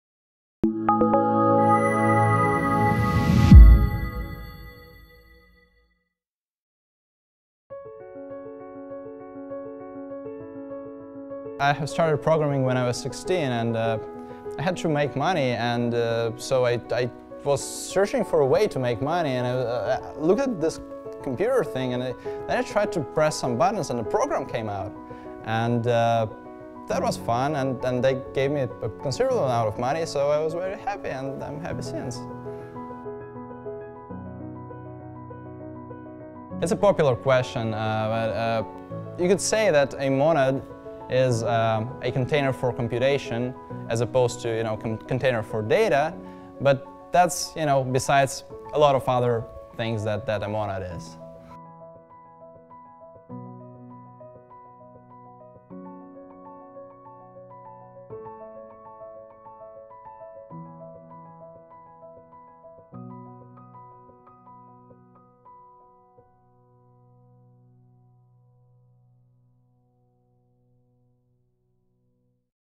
Jonn Mostovoy, IOHK Serokell, Lead Developer - Monads.
Jonn Mostovoy loves mathematics and social sciences; he has ten years of experience in industrial programming. In the spare time Jonn likes to think about and discuss matters of societal structures and governance models, along with the matters of alternative societies and self-governing entities.

In his work, Jonn Mostovoy performs duties of technical management of Serokell team, which is the core developer team of IOHK, industrial research, and programming.

Jonn Mostovoy is an advocate for the wide adoption of functional programming languages, using mostly Haskell as a reasonable trade-off between industrial requirements and type system expressiveness.